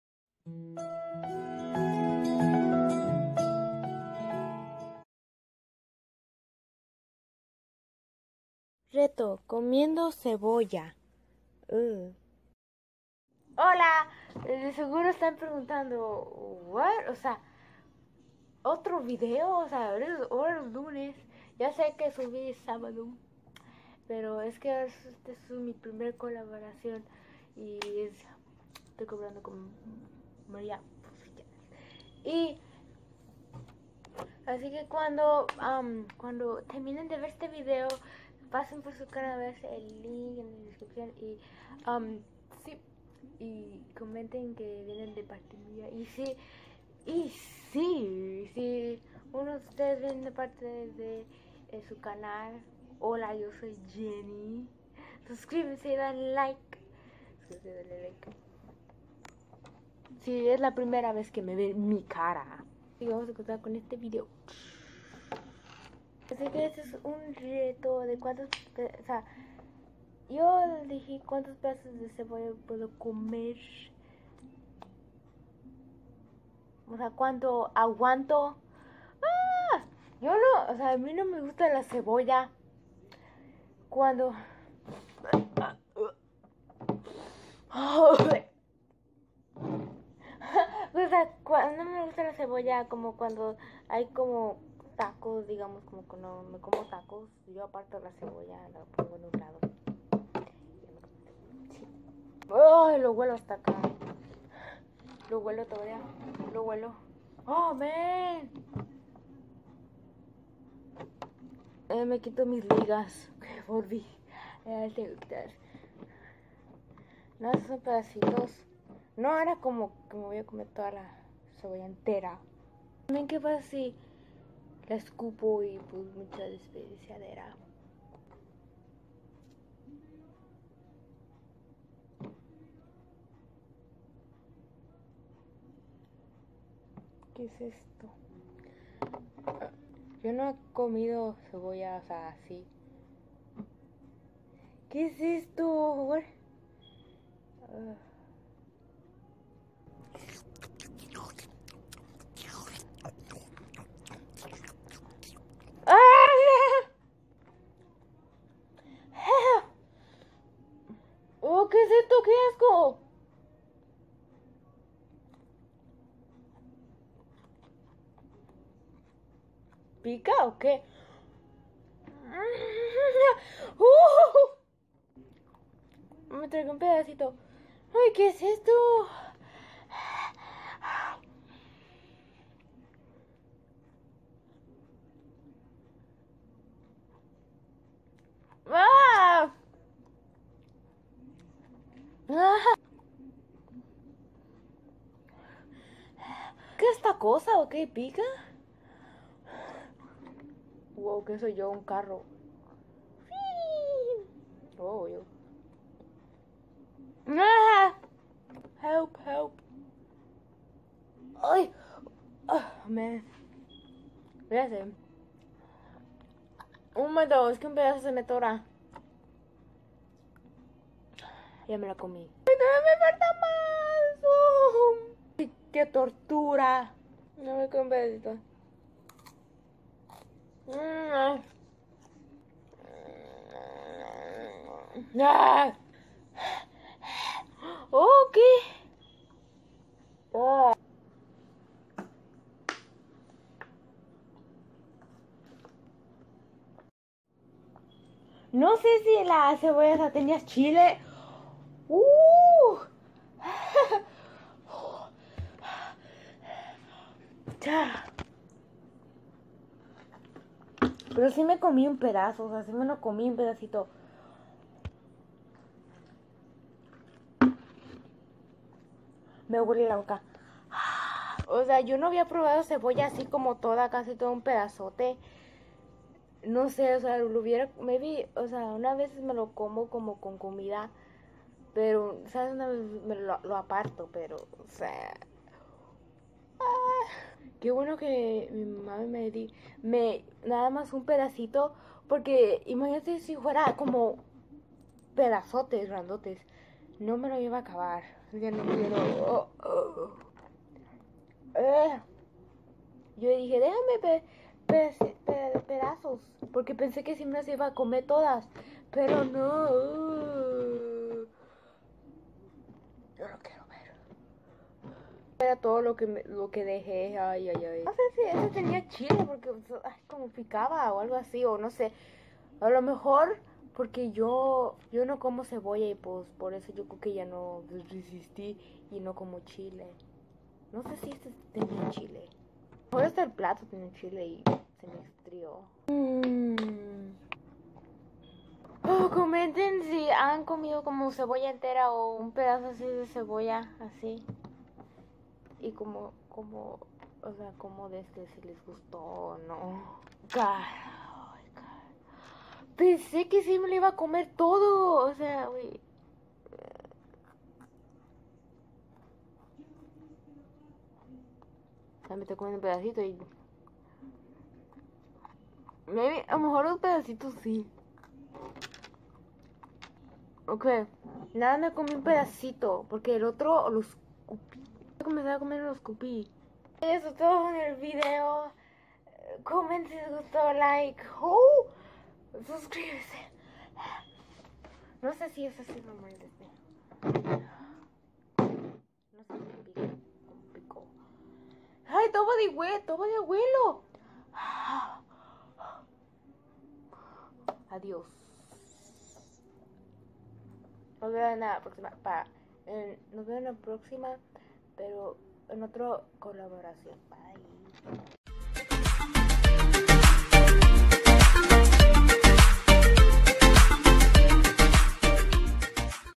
RETO comiendo cebolla | ft. Maria Posillas
Holaa este es una colaboracion
Mi primer colaboracion

NO SUBIO SU VIDEO

Adios 😁👋✌😘muaa